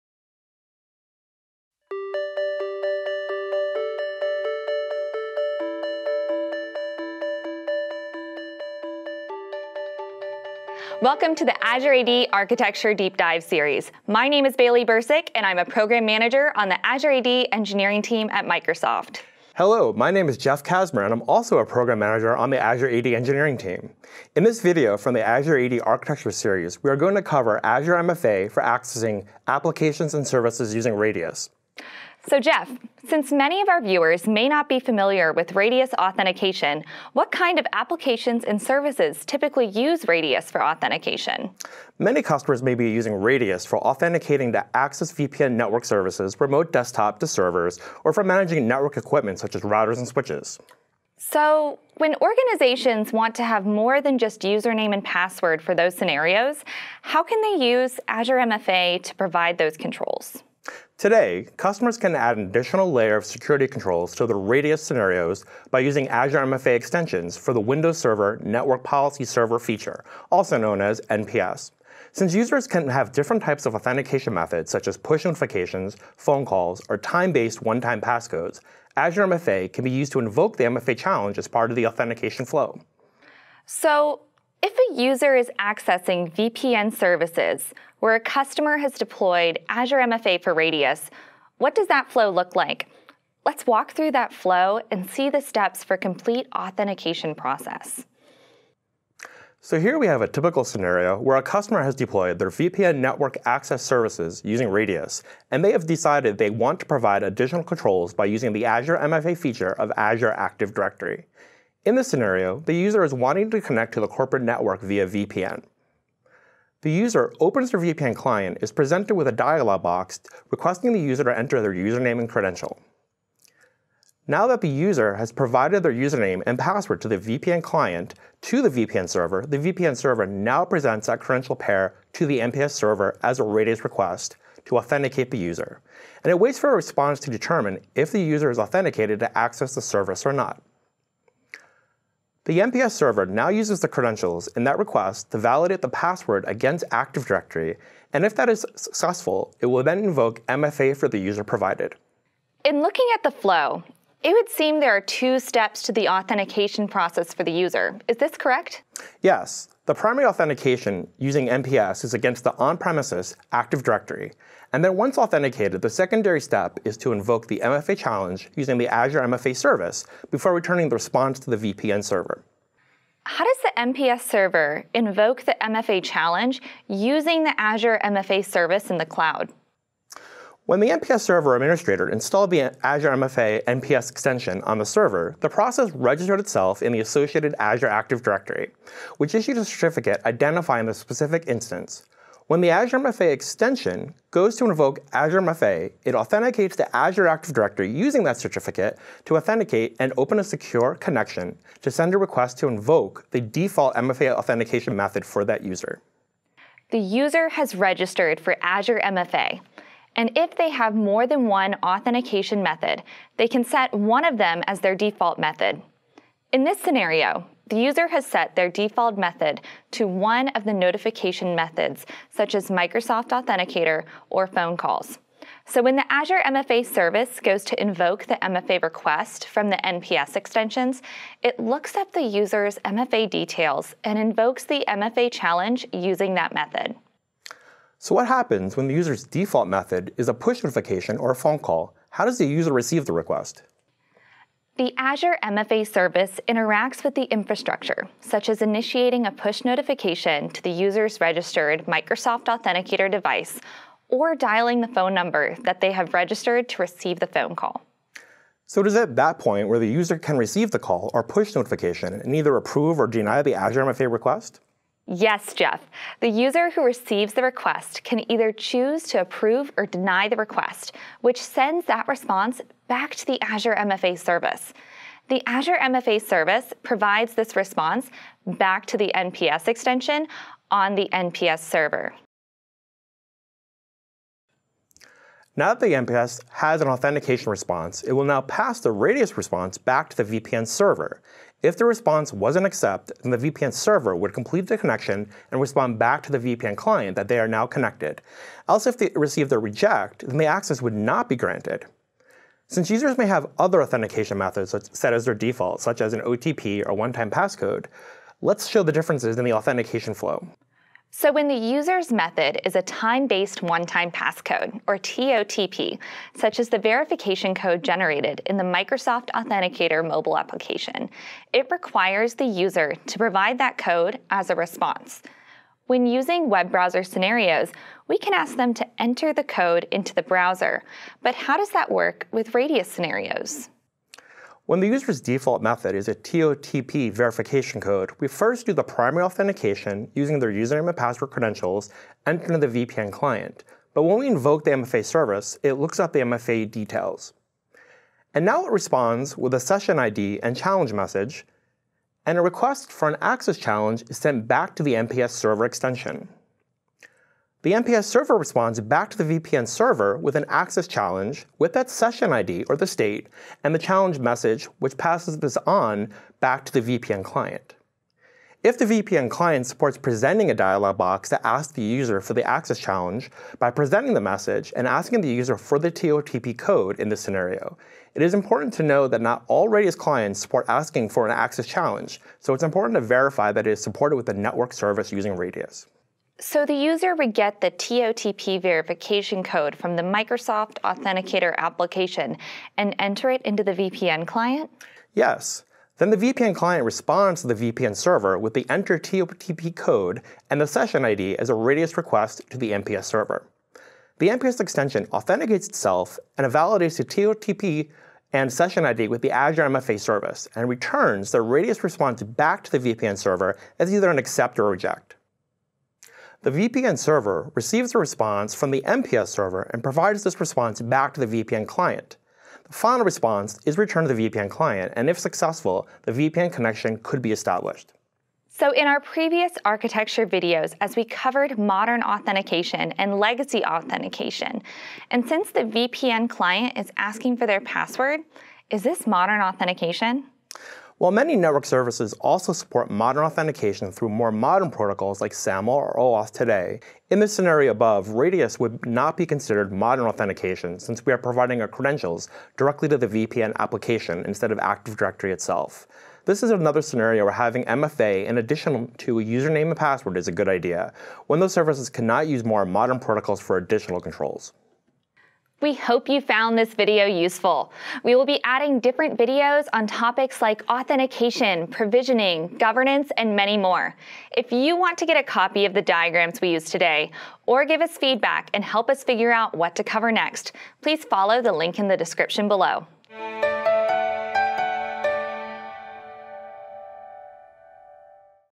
Identity Architecture: MFA with RADIUS | Microsoft Entra ID
In this video, learn about using Azure Multi-Factor Authentication (MFA) for accessing applications and services using RADIUS.